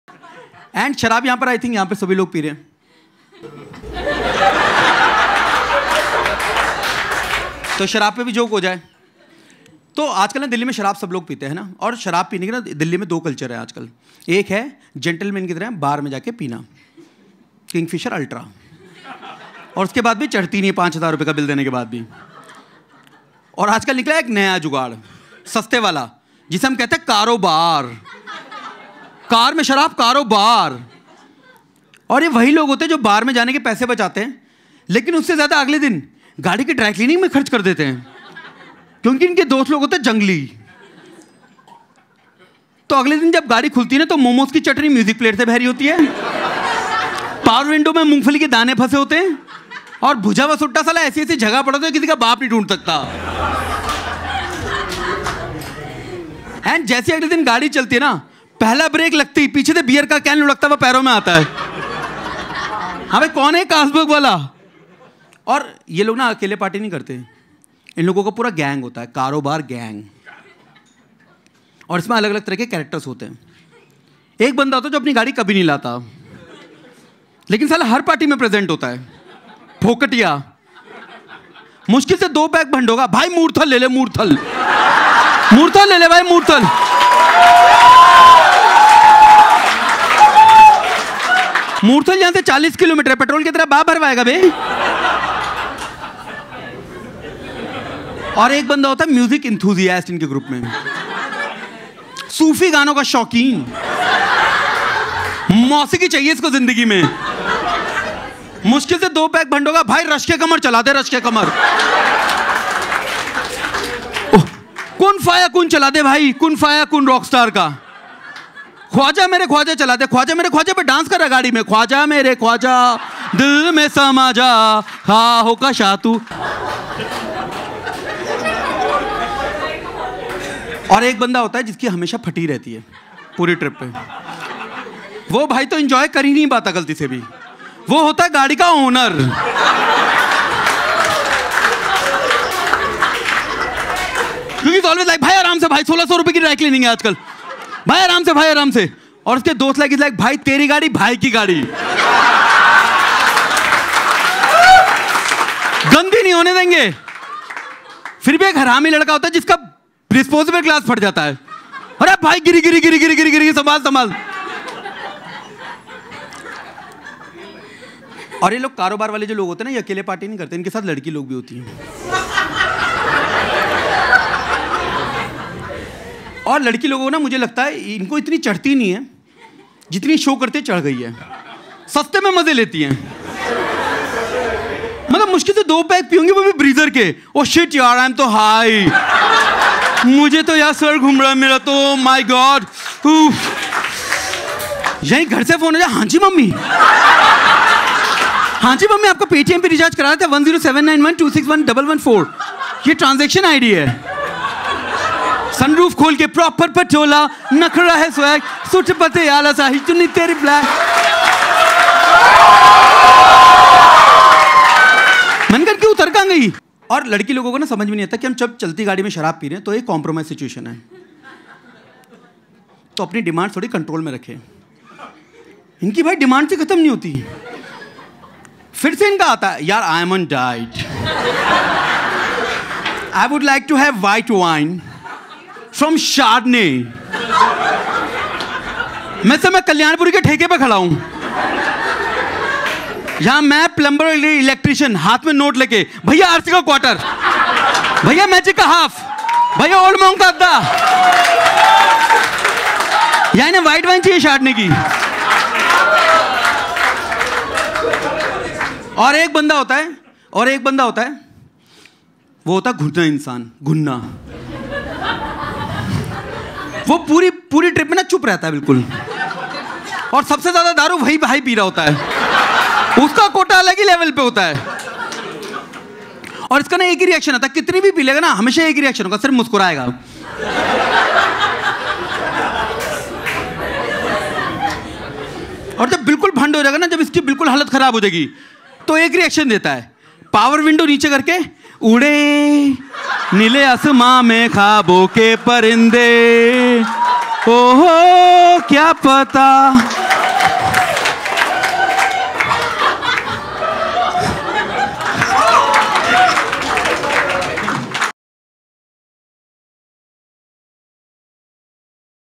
Car-O-Bar |Stand up comedy by Gaurav Gupta
This stand up video describes funny incidents and tales of new drinking culture called Car O Bar which means drinking in the car with  friends.

For show bookings and queries, pls email at

Credits:-
Video recording and editing:- Akshit Arora
Shot at:- Canvas Laugh Club, Gurgaon
Written and performed by:- Gaurav Gupta

Special thanks:- Ankita Jhalani, Manish Tyagi and Pooja Goel